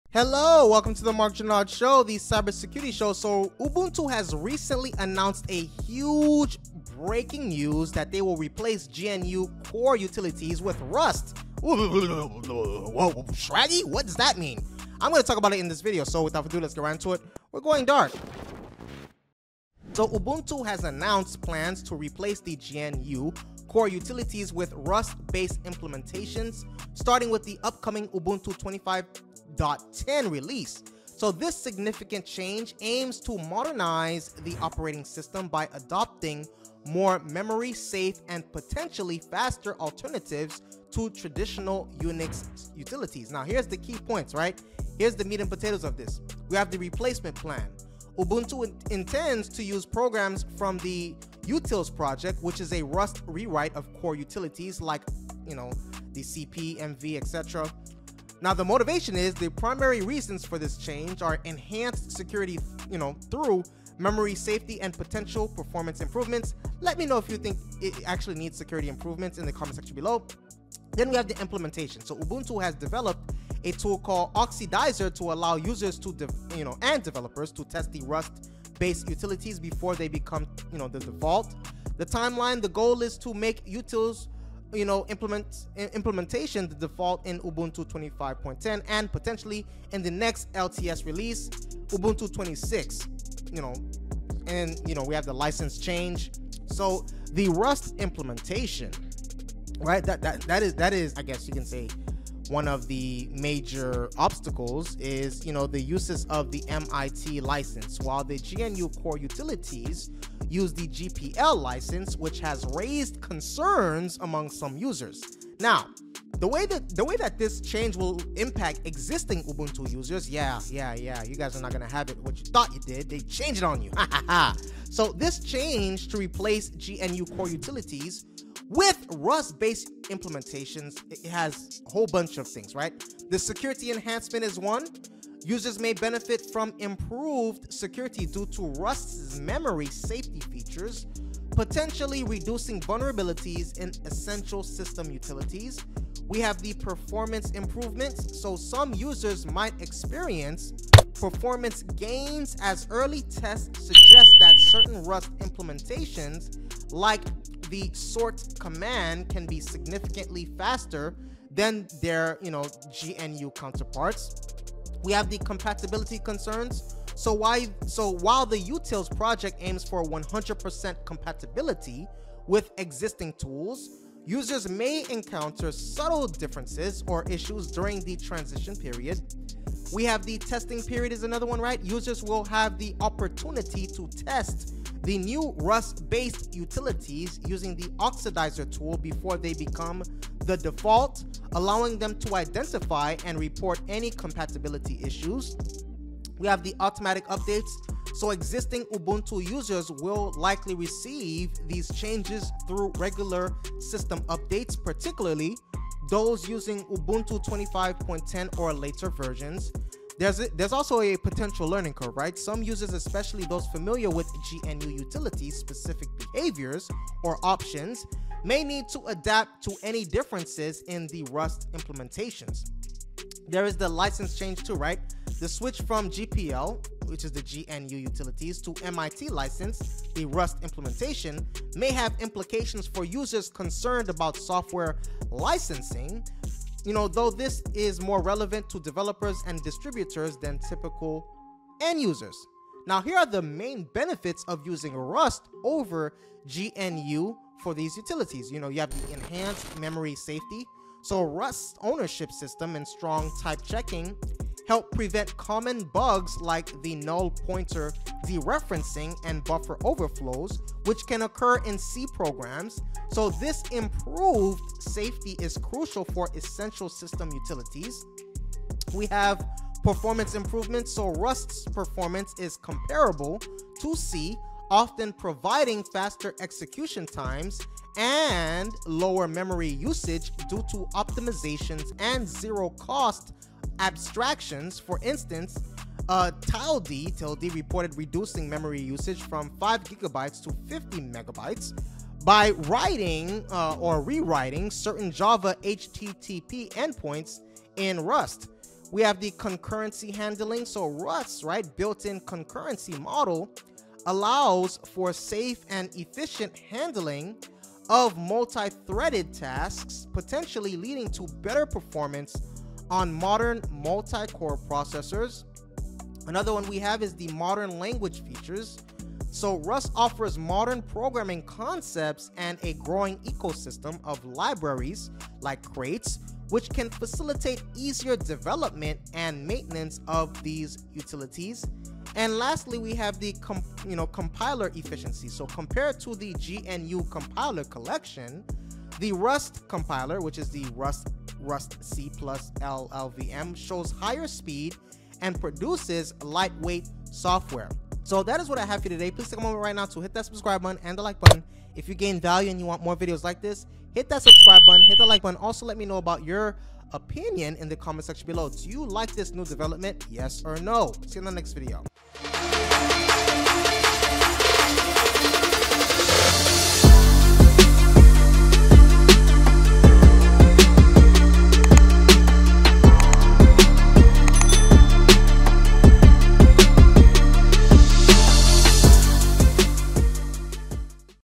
Ubuntu Is Going To Replace GNU Core Utilities With Rust
Ubuntu has decided to switch to using Rust implementations of core utilities. In this video, I breakdown what this can mean for the broader Linux community. This change will be implemented in the upcoming Ubuntu 25.10 release. ls, cp, and mv are among the programs Ubuntu wants to use from the uutils project. Oxidizr was created by Ubuntu to provide users with the capabilities to test rust-based utilities before they become the default. MIT license is what is used by Rust while the GNU core utilities uses the GPL license.